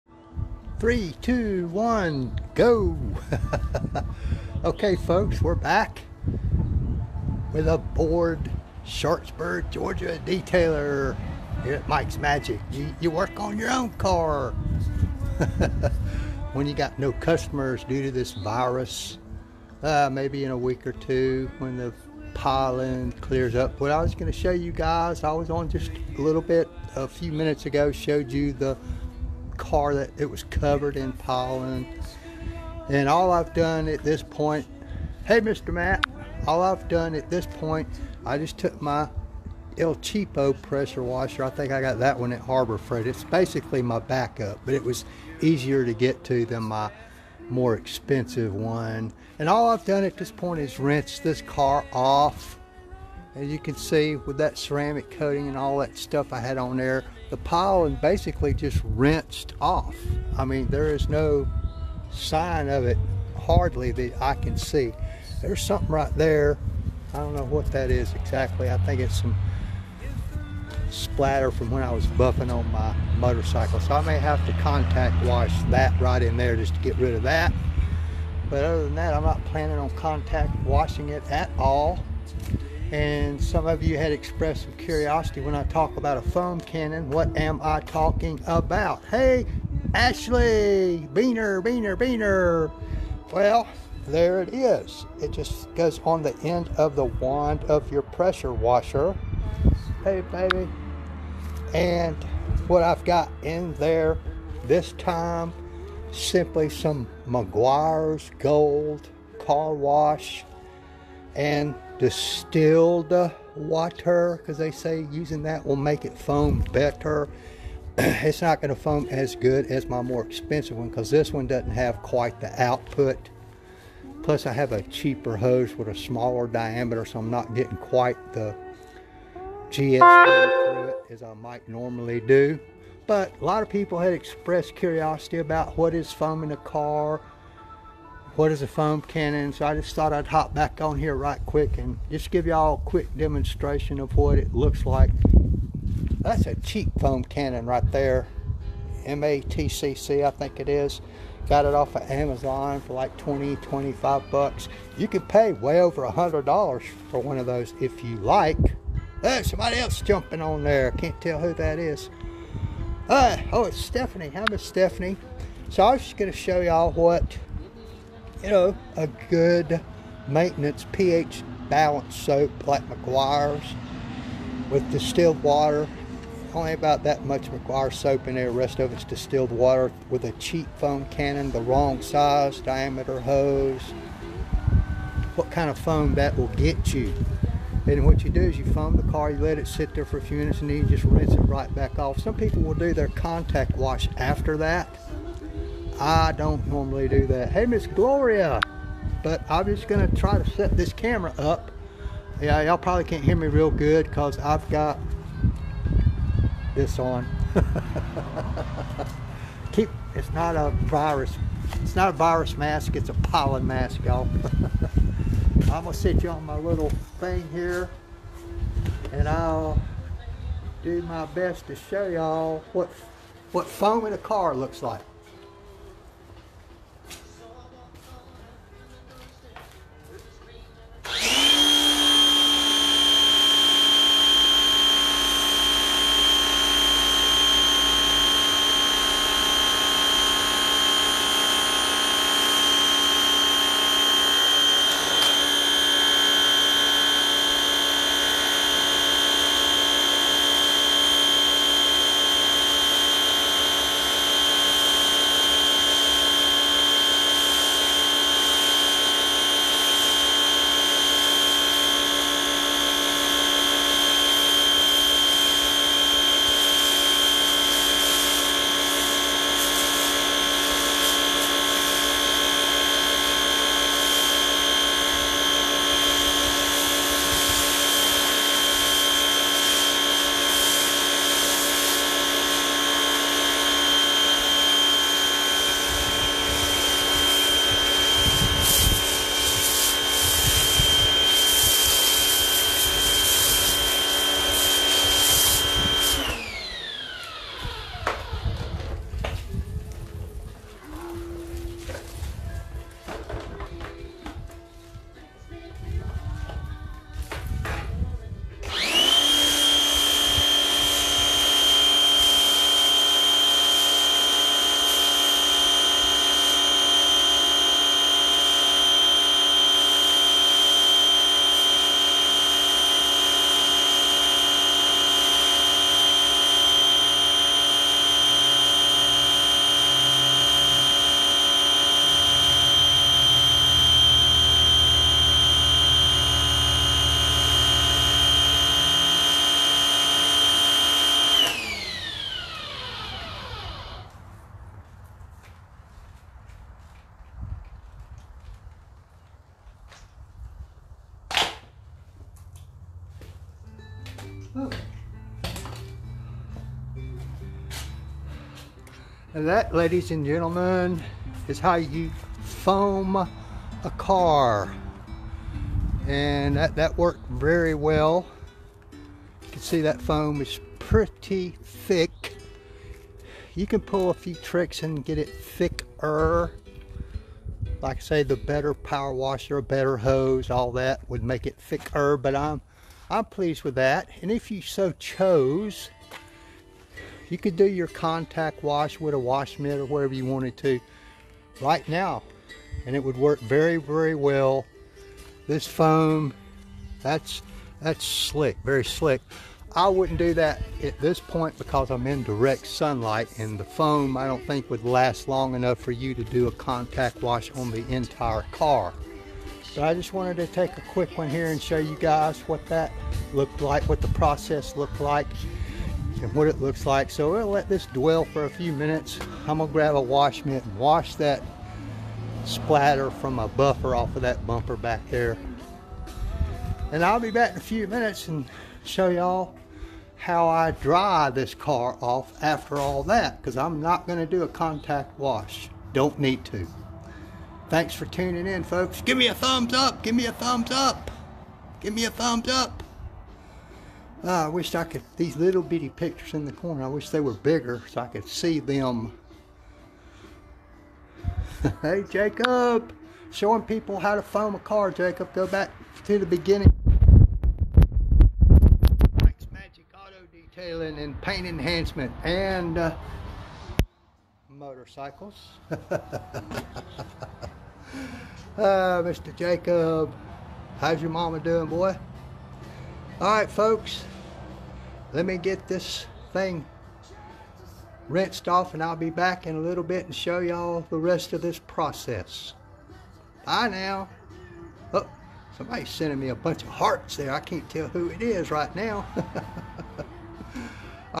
pollen removal part2
working on removing pollen from My personal car, in some down time.  NO contact wash